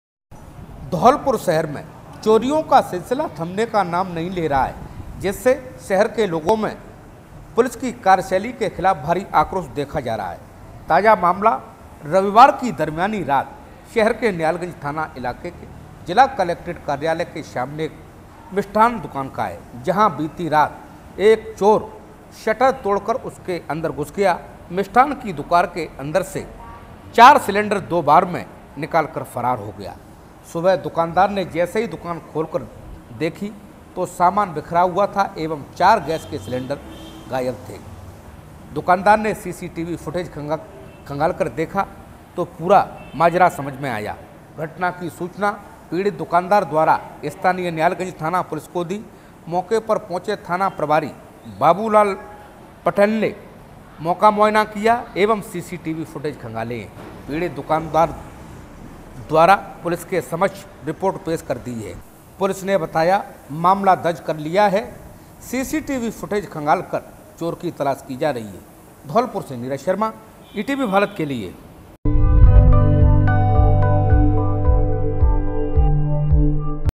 महंगाई और त्योहारी सीजन में हलवाई पर मार | 4 cylinders stolen from sweet shop in dholpur
धौलपुर कलेक्ट्रेट कार्यालय के सामने मिष्ठान की दुकान से चोरों ने चार सिलेंडर की चोरी कर ली. चोर शटर तोड़कर दुकान में घुस गए और चार सिलेंडर चोरी हो गई. चोरी की वारदात में सीसीटीवी में कैद हो गई है.